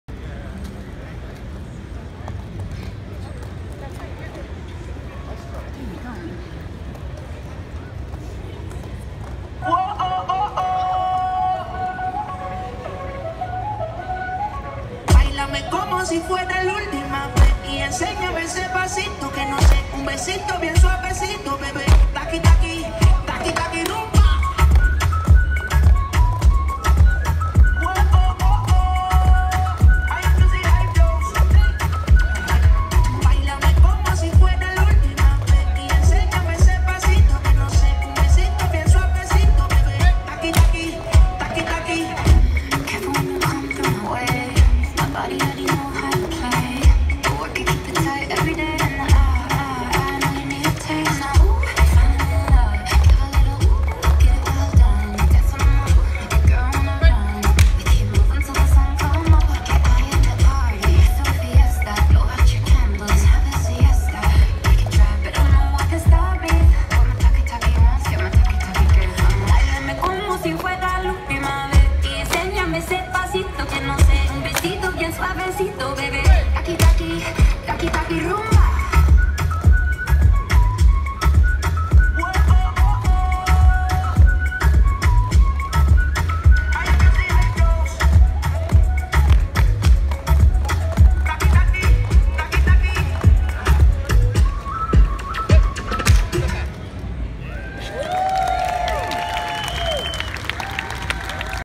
Cha Cha on Roller Skates
The Old School Skaters performing "Cha Cha" on roller skates at the San Diego County Fair.